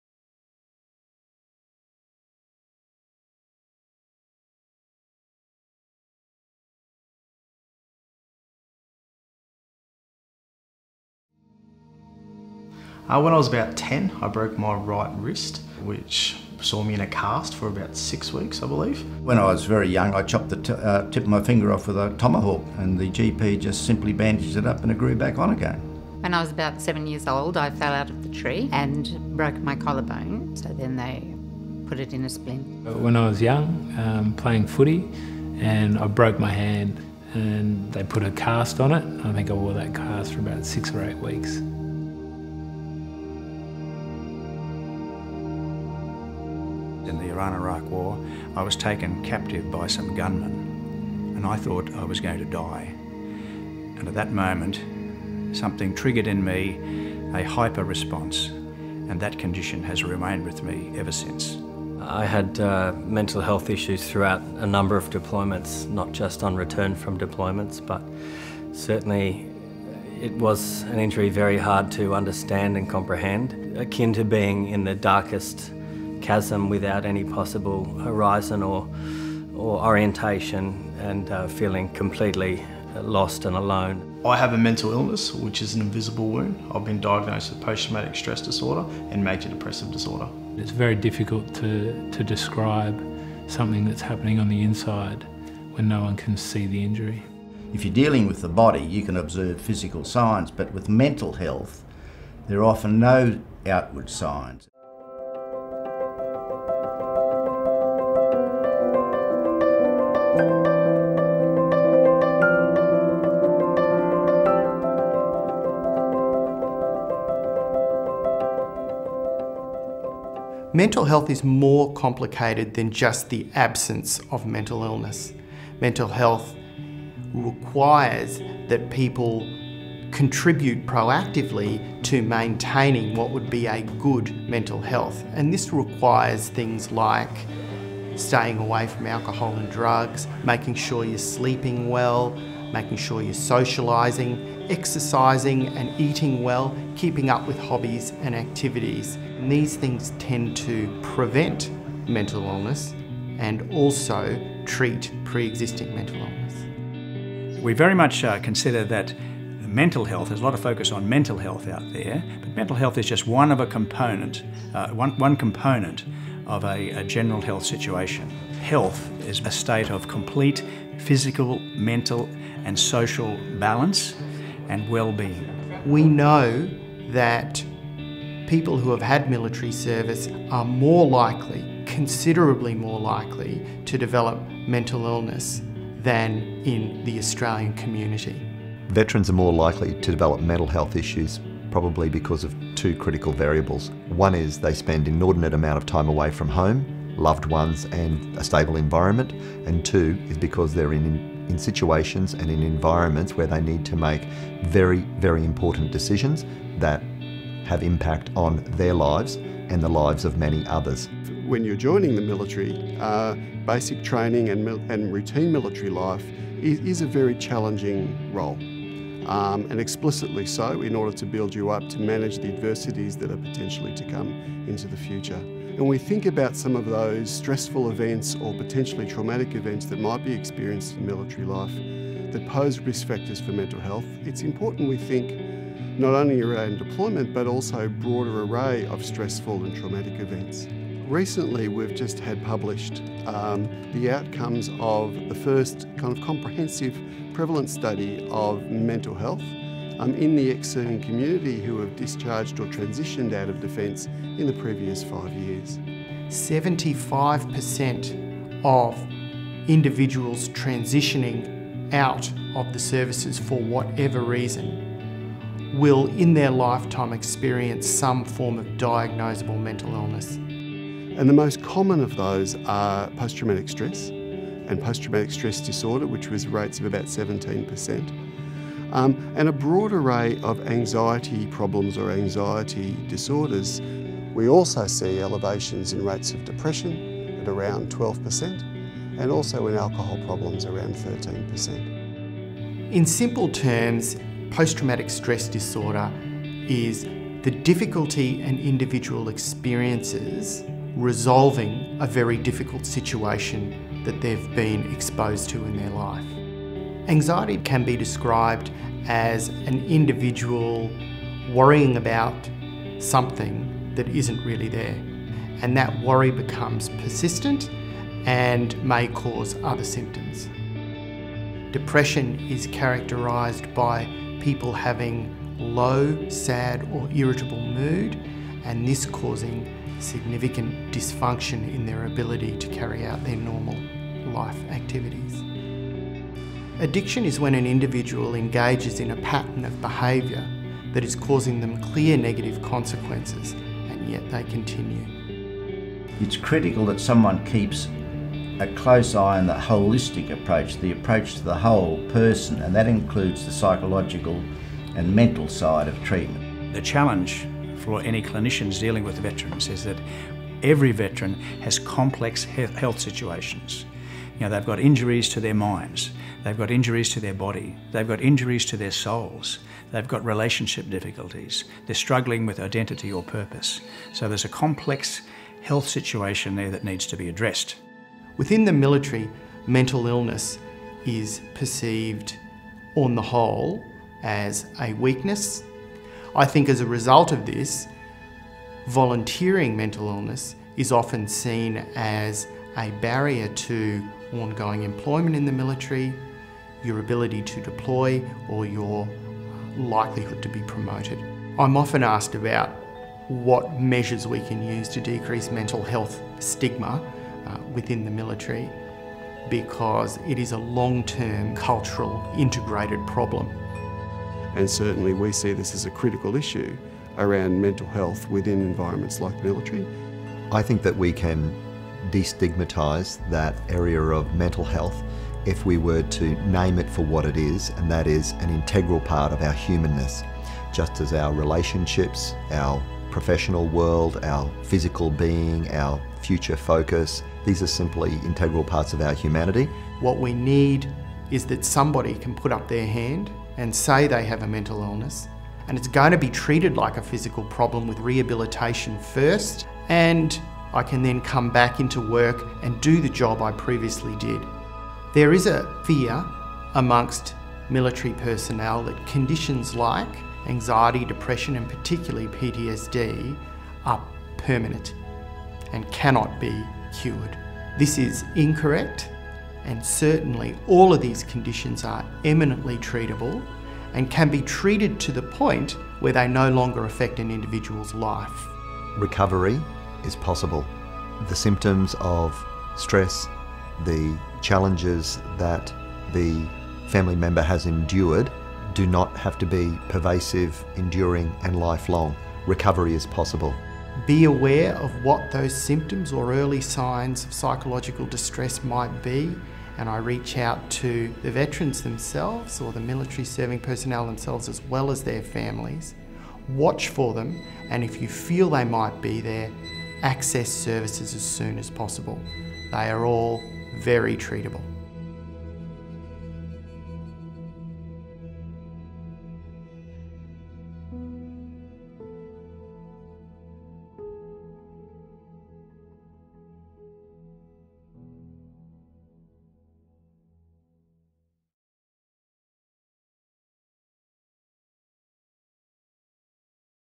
Veteran Family Mental Wellbeing Series - Episode 2: What is Mental Health?
Ex-service people and their families can face a range of mental health challenges, but good mental health is about more than just the absence of mental illness. This video features interviews with experts in the field of Veteran mental health, including Dr Andrew Khoo and Professor David Forbes, who provide commentary on the key contributing factors and symptoms Veterans may experience when it comes to mental illness.

The Bolton Clarke Group, including Altura Learning and The Bolton Clarke Research Institute present the Veteran Family Mental Wellbeing Series.

This a series of educational videos about mental health conditions associated with returned service people, with an emphasis on PTSD.

See more of our content at alturalearning.com